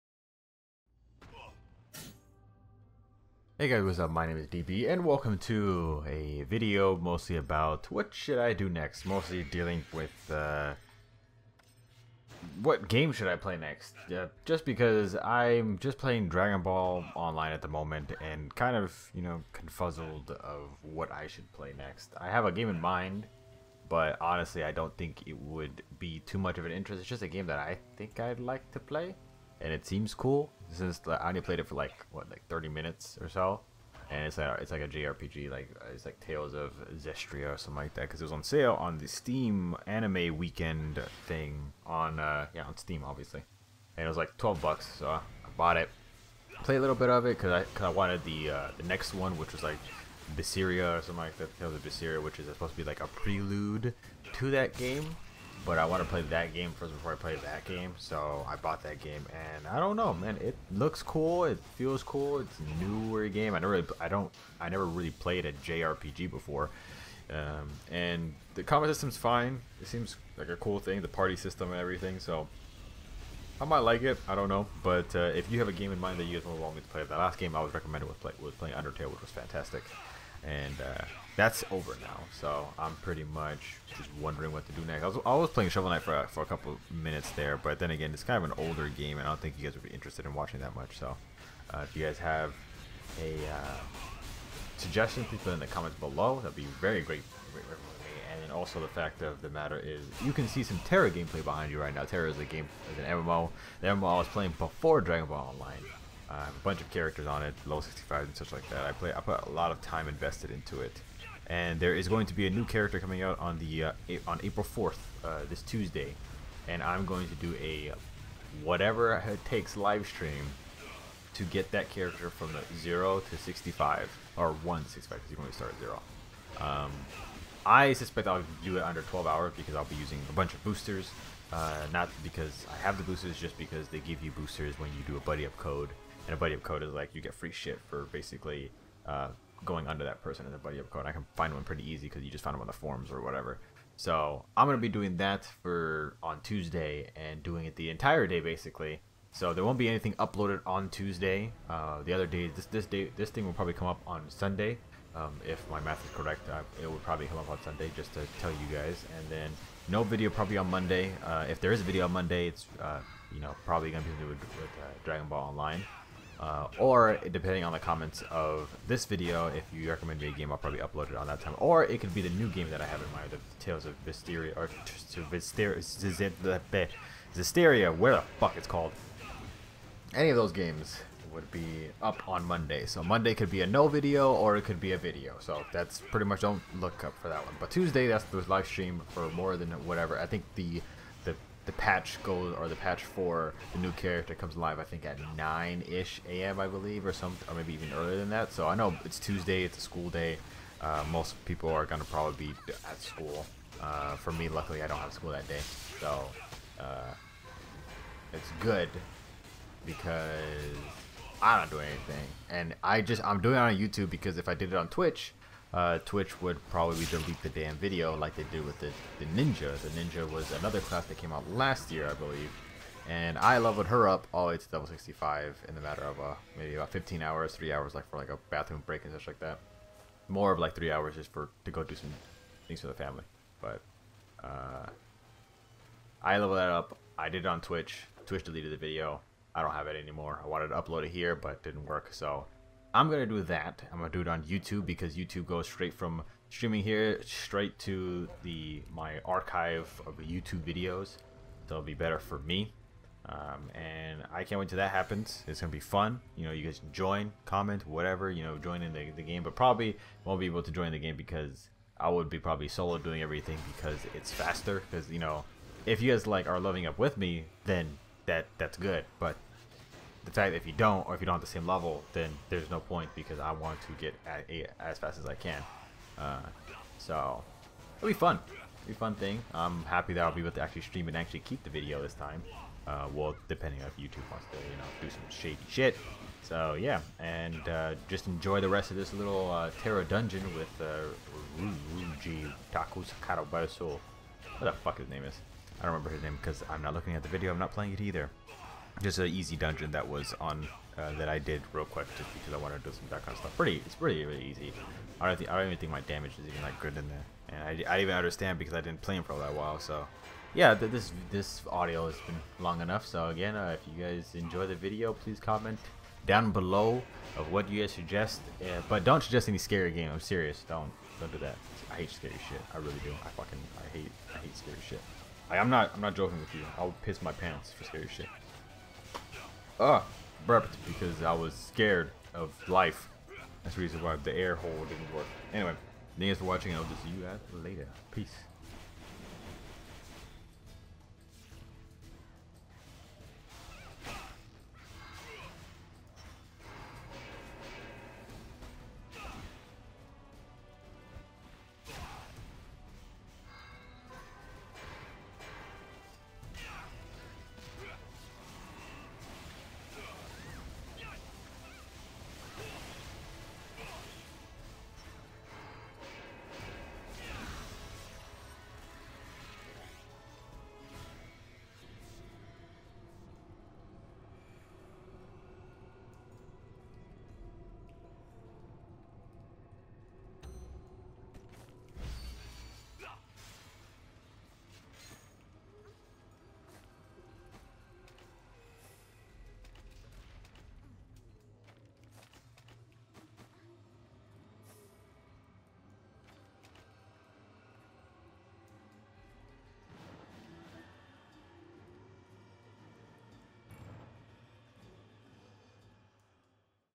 What game should I play next?
-Tera Stream on April 4th
-Comment your game suggestion down below!
Well. If you dont I will just keep doing that I think is best. Which doesnt seem to be going well?
xD

Thanks!
:D